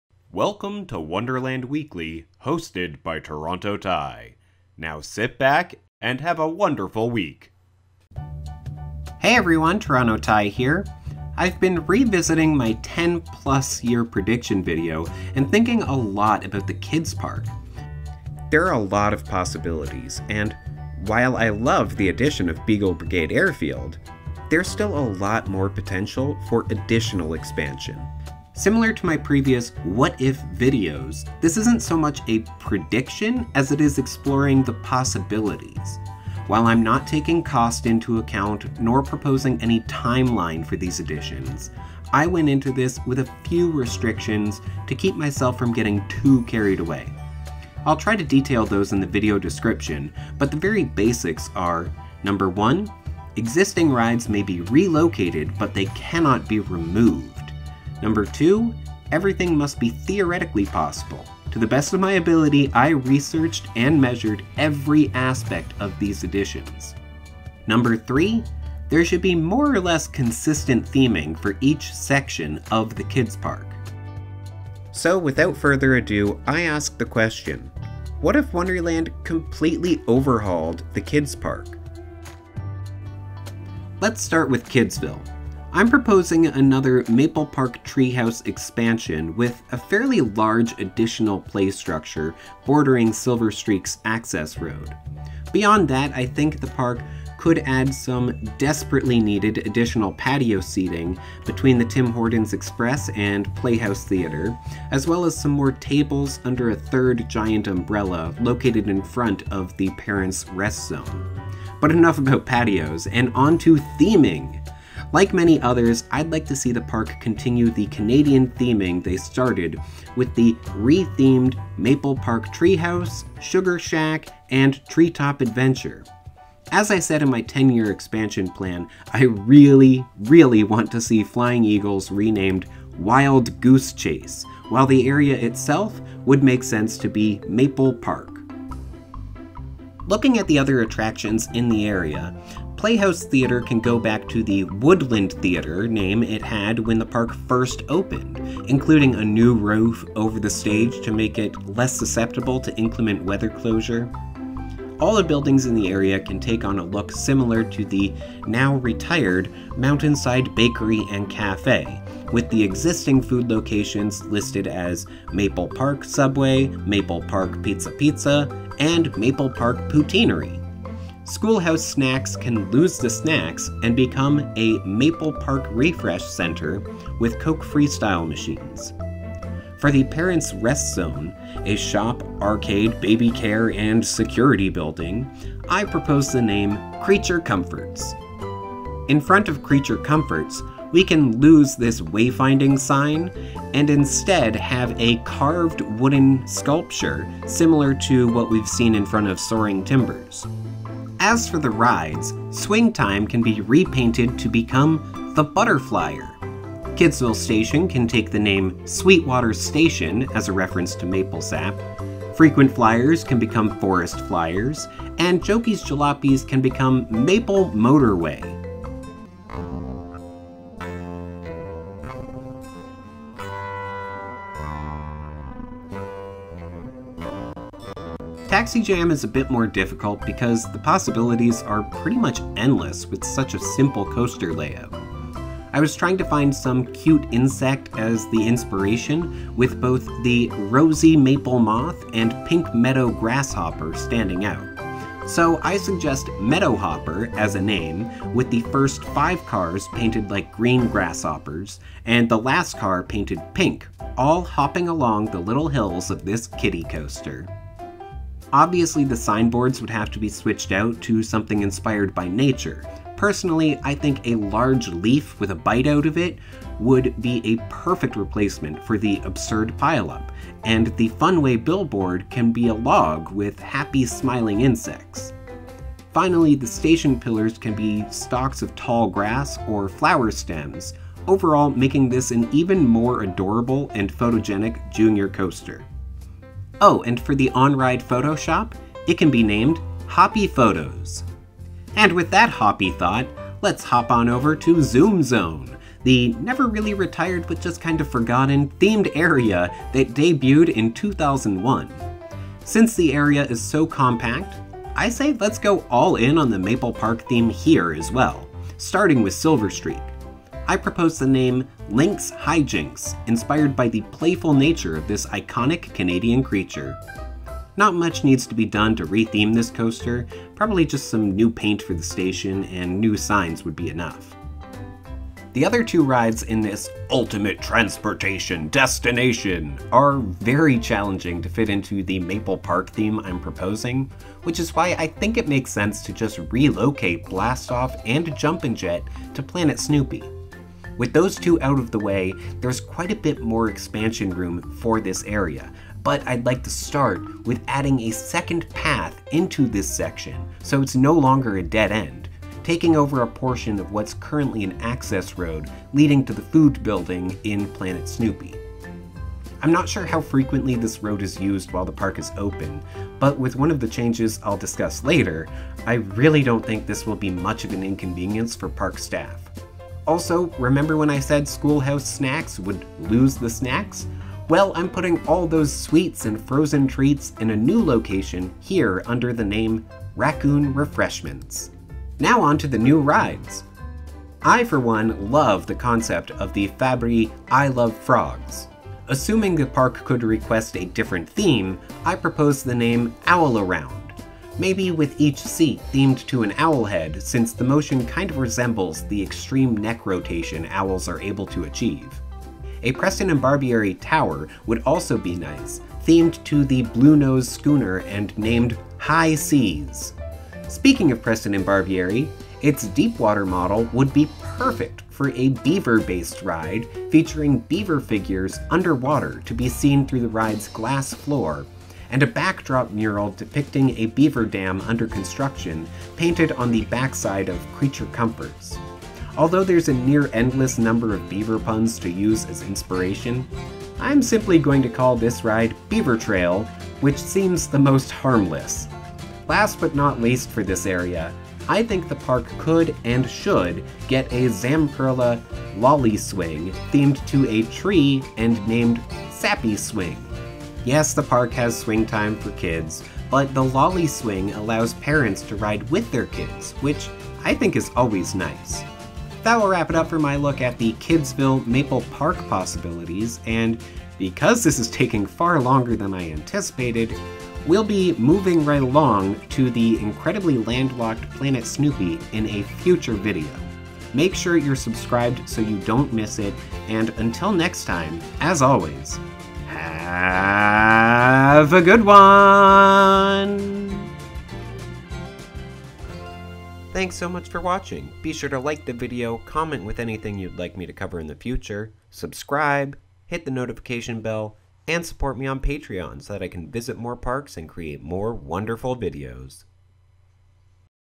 Retheming KidZville! | What if Wonderland Overhauled the Kids’ Park? (Part 1)
Canada's Wonderland has one of the largest kids' parks of any amusement park in the world, featuring a total of 25 rides across three themed areas, a large playground, a 500 seat open-air theatre, an immense amount of natural beauty, and plenty of potential to be even bigger and better!

First in a three-part series, this video takes a close look at the existing KidZville and Zoom Zone areas, addressing everything from patios and pathways to brand new rides and theming elements!

Apologies for the inconsistent audio quality. I was too far into developing this video to correct it, but I plan to re-record the audio for part two and three to ensure they maintain consistent sound quality.

IMPORTANT: This is absolutely not a prediction! Although I would love to see all these changes come to the park, I'm not so bold as to think this is in any way likely to happen. Some of my proposed changes may come to be, but I am by no means an expert and trust the park's decision-makers to plan and develop the park to be the best it can be.

RESTRICTIONS: These are the main guidelines I used to focus my ambitions.

1) Rides cannot be removed! - I firmly believe the park still has plenty of room to add new rides (and other projects) without removing any existing rides. The park needs all the capacity it can get, so as long as it can add without removing, it is guaranteed to make for a more enjoyable day for guests. Having said all that, sometimes a ride can be in such a location as to shut off access to additional expansion space. In these instances, I believe the park is better served to relocate or otherwise adjust the existing ride to make room, as was seen when the park relocated Swing of the Century to make room for Racing Rivers, and more recently when Wonderland adjusted Treetop Adventure's layout to secure the proper ride envelope for Flying Eagles.

2) Everything must be theoretically possible! - Oh, boy, does this ever limit what I was able to do! I researched everything from ride footprints (both through the ride manufacturer's official websites whenever possible and through careful Google Earth measurements) to the Ontario fire code (though I had trouble learning much from the "legalese"). Ultimately, I don't know what infrastructure the park has or is able to add, nor do I know what seemingly simple thing could actually be prohibitively expensive. However I want to be clear that I did my absolute best to ensure everything in this is theoretically possible. When in doubt, I erred on the side of caution, so there is a certain margin for error on most things I'm proposing.

3) Consistent theming! - My biggest frustration with KidZville in its current state is how disjointed everything feels. Some of it is a city made for kids, but other parts are clearly themed to Canada and its natural elements. To really illustrate the differences between themed areas, this miniseries is broken down into three segments, each addressing a specific themed section of the kids' park. There are times where you'll have to squint to see the connections (such as the name "Ghoster Coaster" technically appearing as "PEANUTS Ghoster Coaster" at the ride's entrance) but I think in general I've designed three cohesively themed sections.

4) I definitely took trees and other natural elements (such as grading) into account while developing this plan, but I will make substantial sacrifices as long as the character and general 'feel' of the kids' park is maintained. This isn't just about what I like, though, since there are regulations regarding tree removal specifically (including requirements for removed trees over a certain diameter to be replaced by another tree within the same general area/environment).

5) Sight-lines and crowd-flow! — This isn't Disney Parks level stuff, but I did consider what guests would see when first entering or leaving any given plaza. In addition, I measured out pathway widths, minimized "dead-end" or "no-exit" plazas, tried to reduce "dead-space" where no rides/attractions/shops were located for long stretches, considered how easy it would be to find the nearest patio/bench, and of course measured out both length and width of queue/exit pathways.

6) Maintenance access — One element often overlooked by those of us who grew up designing parks in Roller Coaster Tycoon and the like is how easy it would be for staff to get around the park. I really thought a lot about how vehicles would make deliveries to various buildings, how many access points from the perimeter road the park would want/need, and even considered how snow plows might travel through the park during WinterFest.